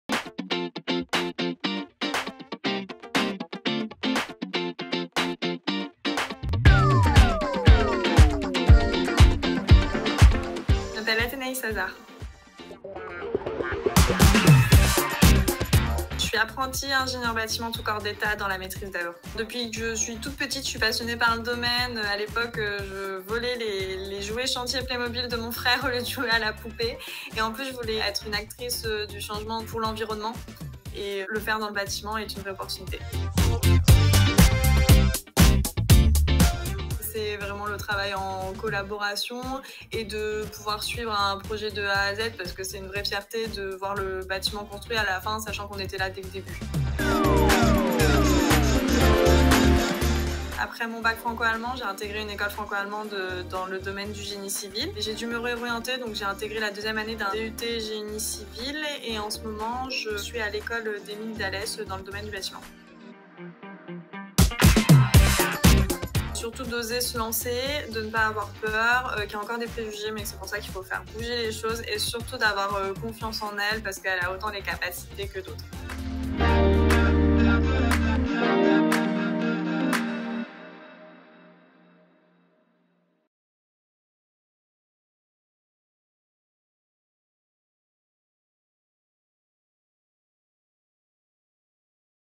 Les Talentes 2021 - Trophée "La Jeune Talente"
Athénais Hazard remporte le prix "La Jeune Talente".

Les Trophées "Les Talentes" sont une cérémonie de remise de prix pour récompenser des femmes aux parcours exemplaires, des métiers, du conseil, de l’ingénierie, du numérique de la culture ou encore du coaching pour ne citer que ceux-là. A travers 7 catégories, un jury composé de professionnels de la prestation de services intellectuels, met en avant des parcours de femmes exceptionnelles qui incarnent parfaitement des valeurs d’engagement et d'innovation.